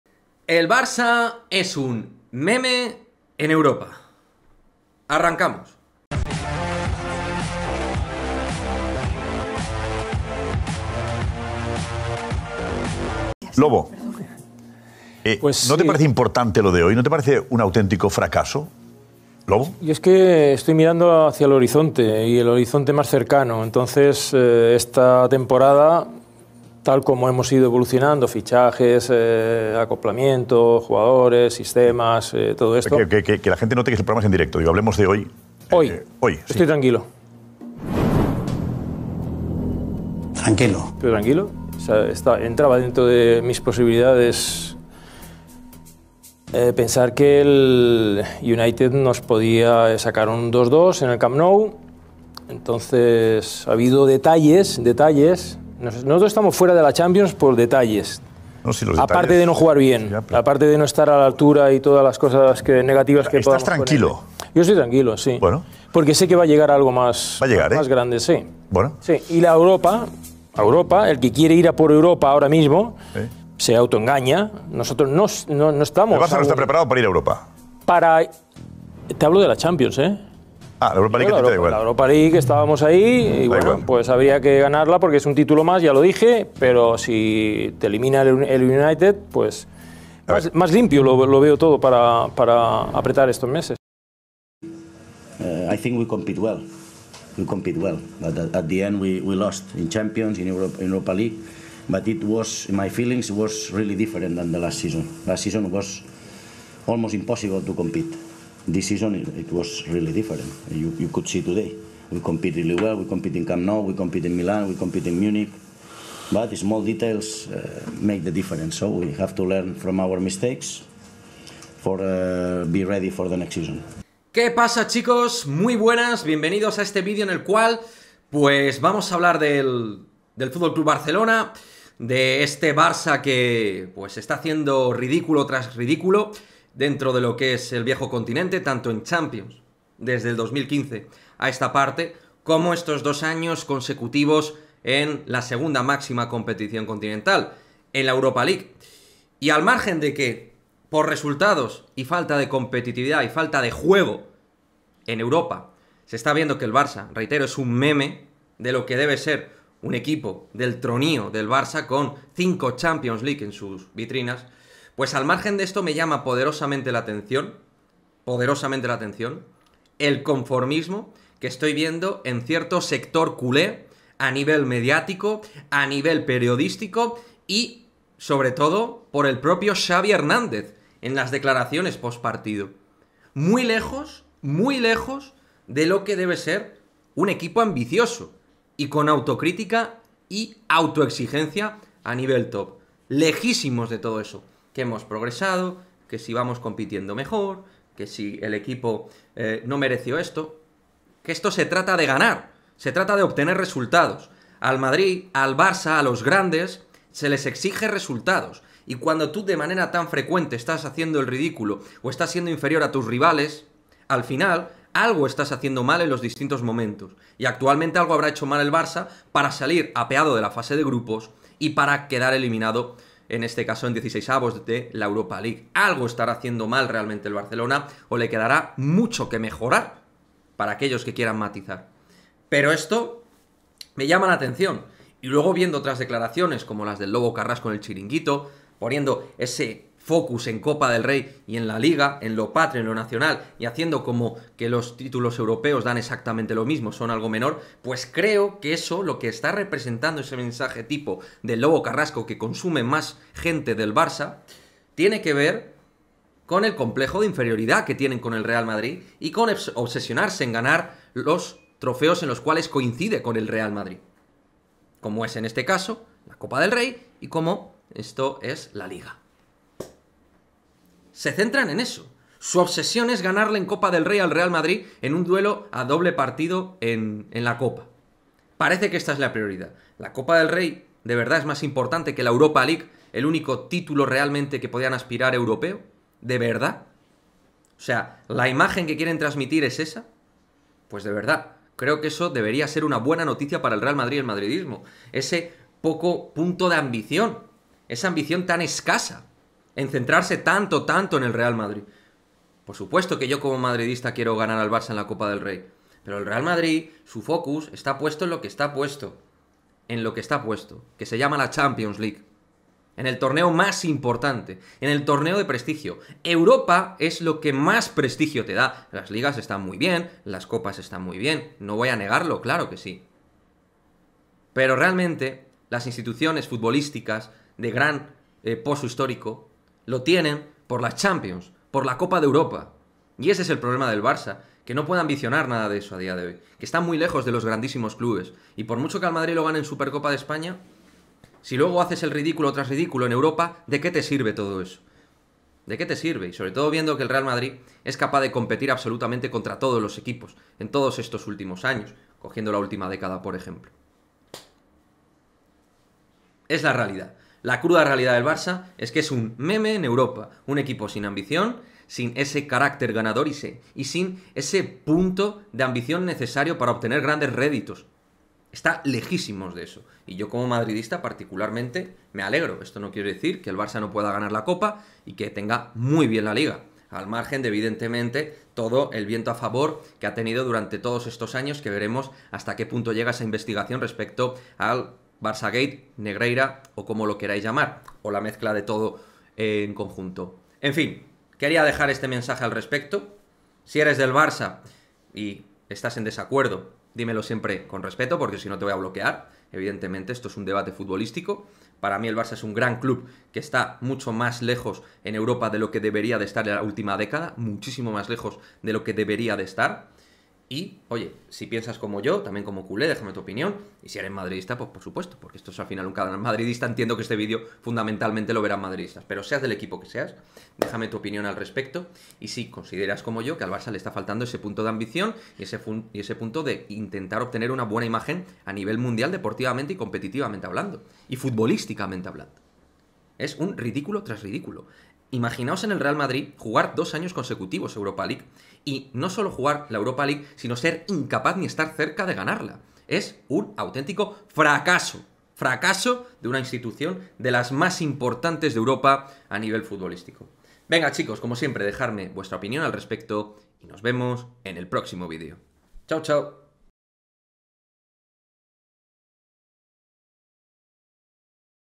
🟢🔥 EL FC BARCELONA es un MEME en EUROPA | RIDÍCULO en la EUROPA LEAGUE 💣📢
En este vídeo la situación del FC Barcelona el cual vuelve a mostrarse ridículo incapaz de ganar a nadie y encima con una falta de ambición brutal.

Un club de la enjundia del Barça debería tener mucha más exigencia y debe mostrarse mucho más autocrítico después de otro fiasco Europeo.

Os mostramos nuestra opinión al respecto de un FC Barcelona que con esta actitud podemos decir que los madridistas nos sentimos más tranquilos.